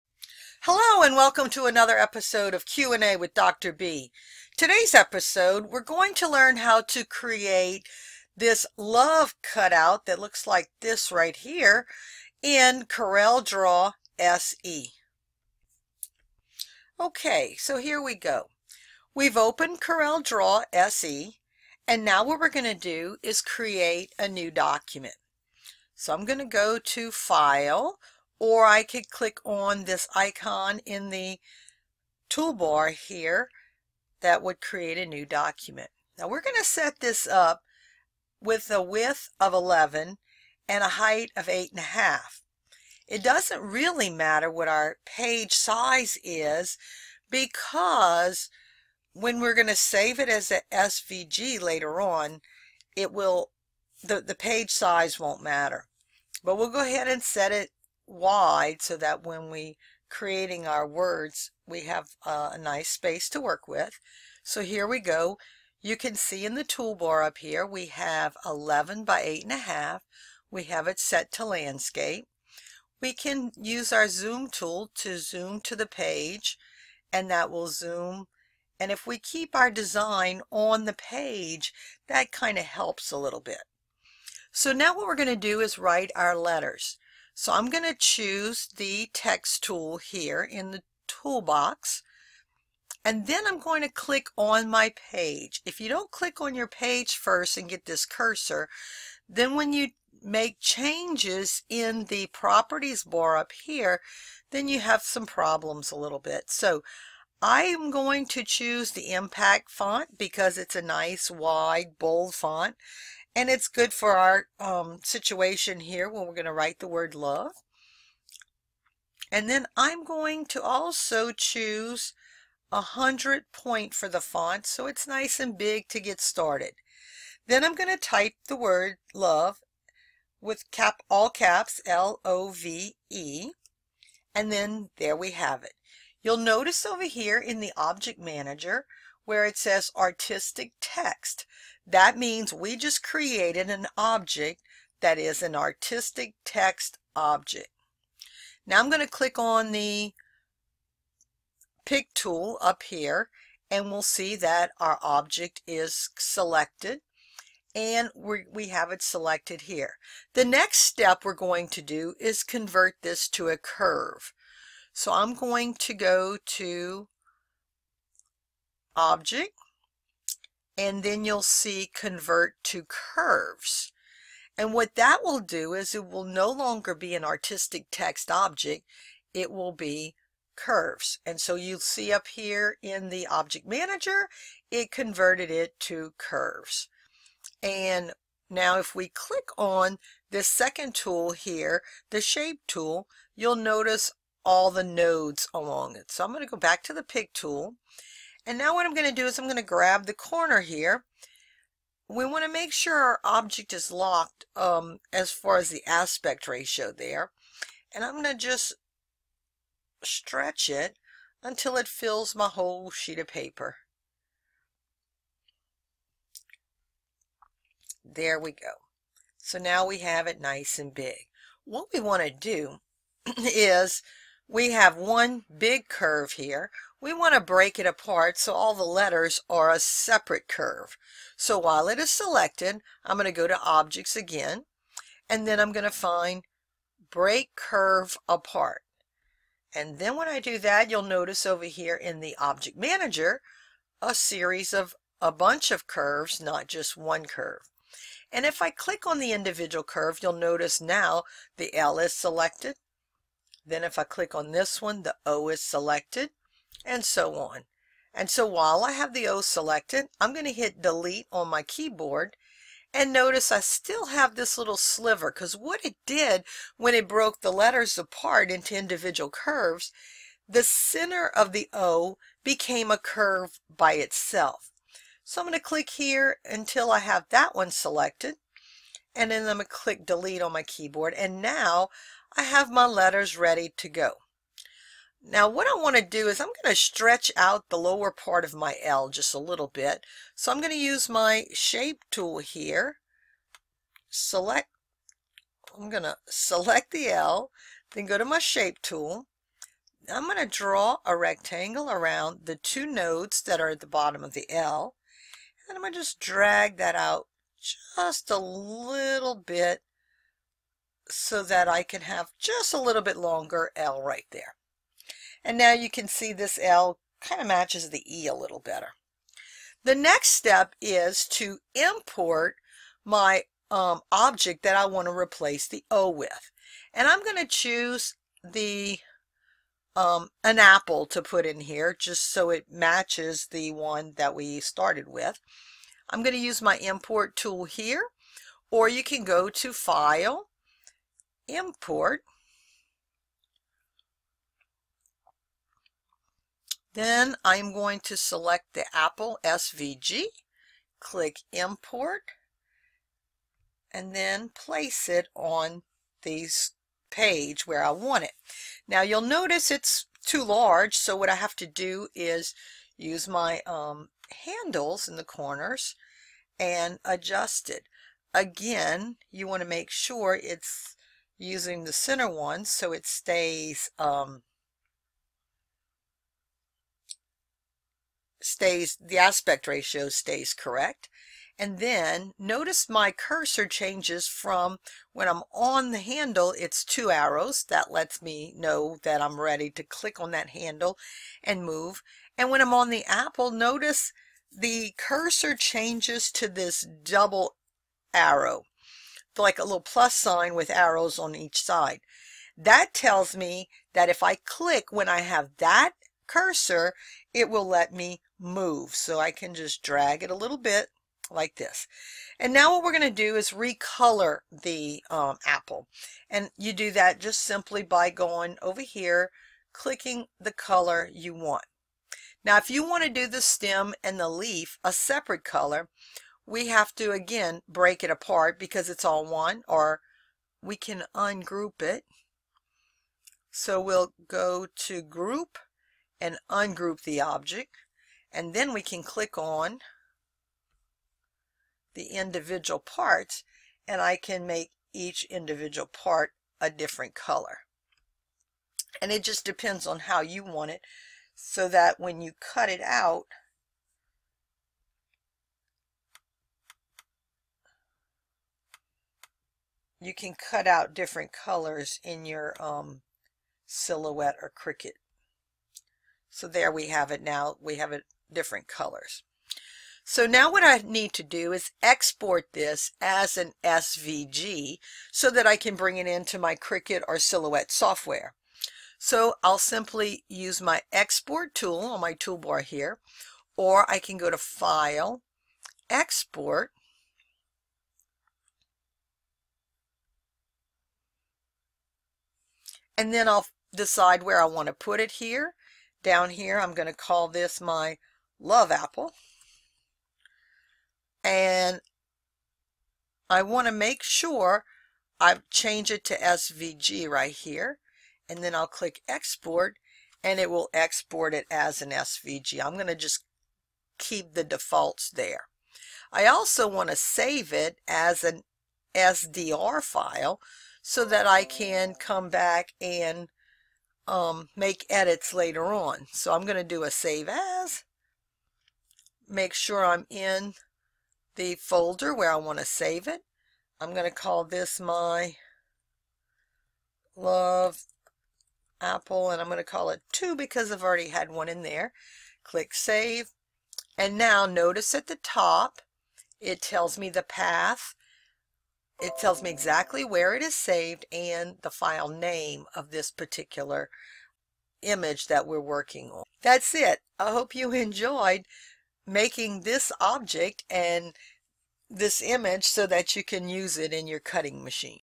How to Create a LOVE Cutout SVG in CorelDraw
Dr. Bee shows how to create an SVG of the word Love with the O replaced with an apple.  This SVG can be used with your Cricut or Silhouette for crafting.